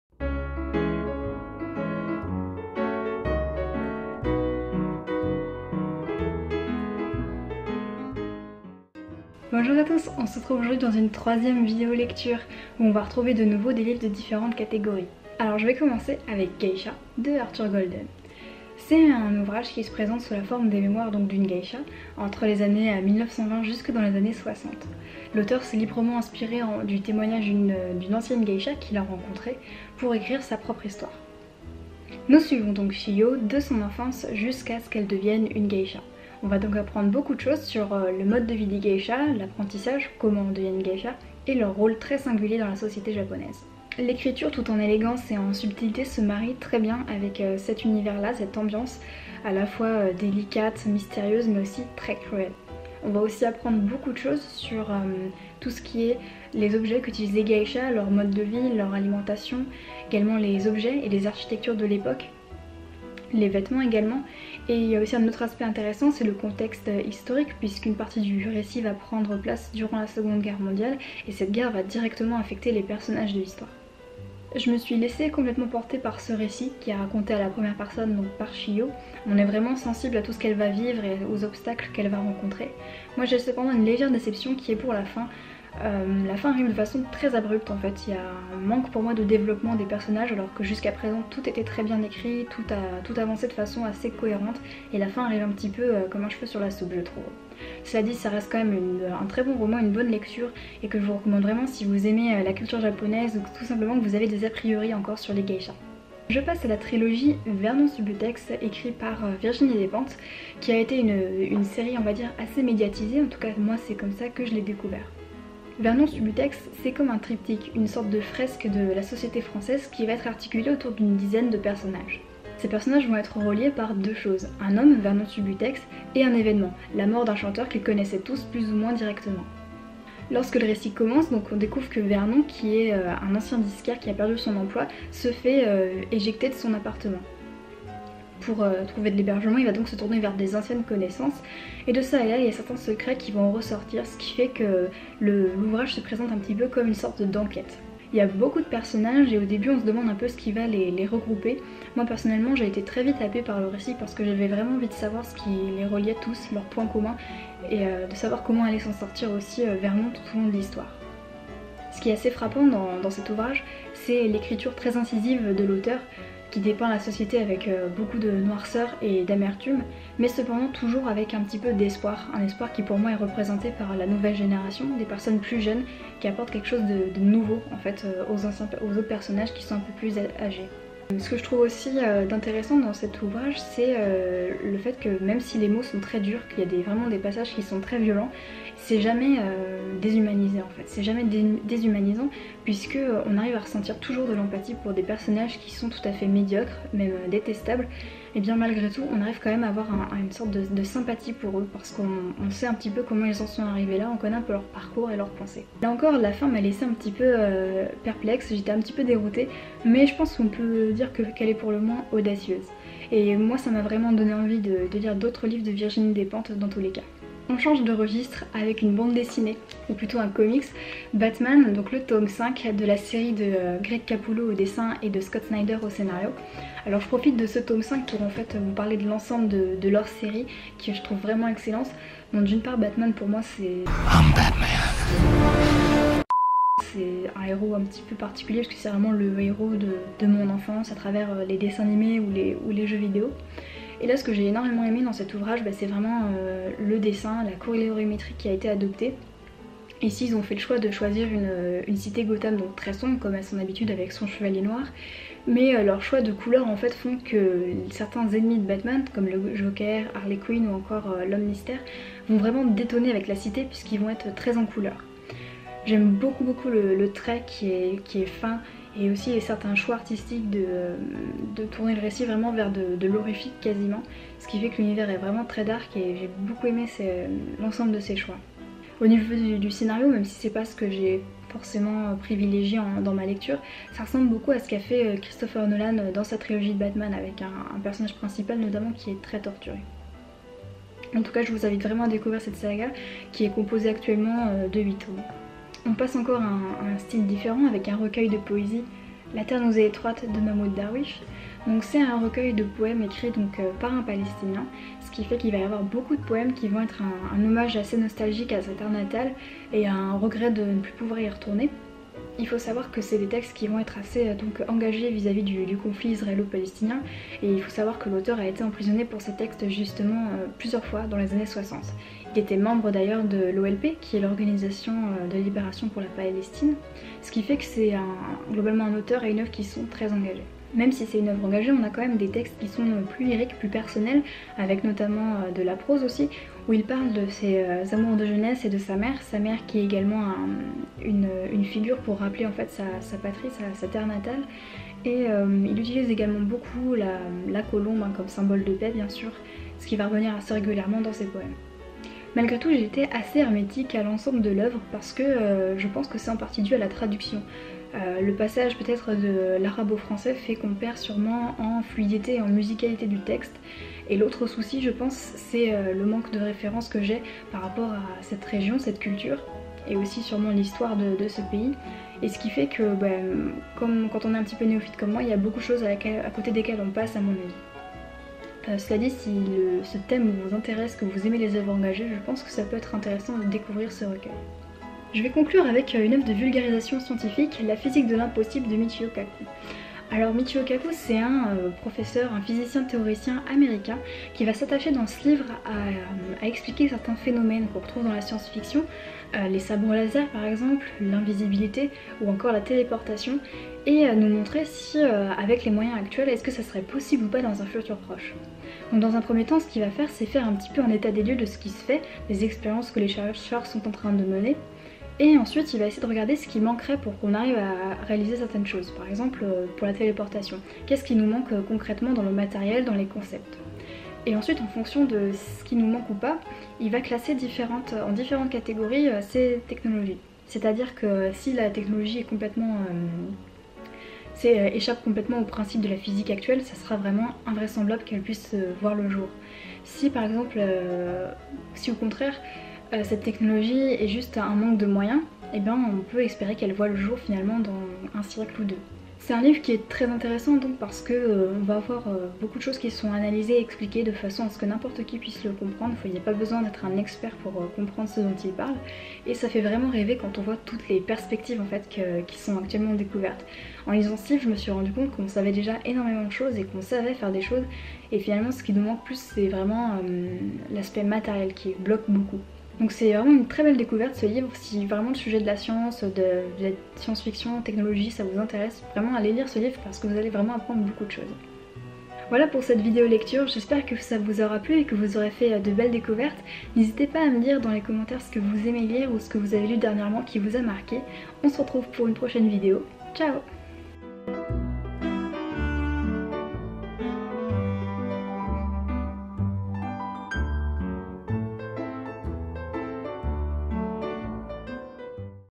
Lectures o2 - L'homme chauve-souris et trilogie de Despentes
Une nouvelle vidéo lecture avec de nouveau des ouvrages divers et variés.

Livres mentionnés :

• Geisha de Arthur Golden
• Vernon Subutex (trilogie) de Virginie Despentes
• Batman Tome 5 (L'An Zéro, 2ème Partie) de Greg Capullo et Scott Snyder
• La Terre nous est étroite de Mahmoud Darwich
• La Physique de l'impossible de Michio Kaku